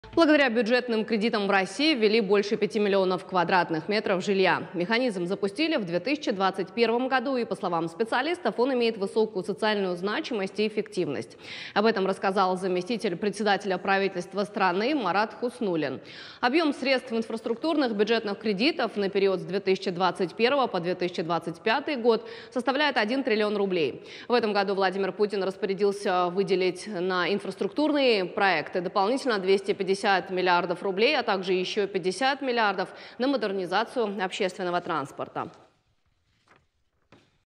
Благодаря бюджетным кредитам в России ввели больше 5 миллионов квадратных метров жилья
Благодаря бюджетным кредитам в России ввели больше 5 миллионов квадратных метров жилья. Механизм запустили в 2021 году и по словам специалистов он имеет высокую социальную значимость и эффективность. Об этом рассказал заместитель председателя Правительства страны Марат Хуснуллин. Объем средств инфраструктурных бюджетных кредитов на период с 2021 по 2025 год составляет 1 триллион рублей.

В этом году Владимир Путин распорядился выделить на инфраструктурные проекты дополнительно 250 миллиардов рублей, а также еще 50 миллиардов на модернизацию общественного транспорта.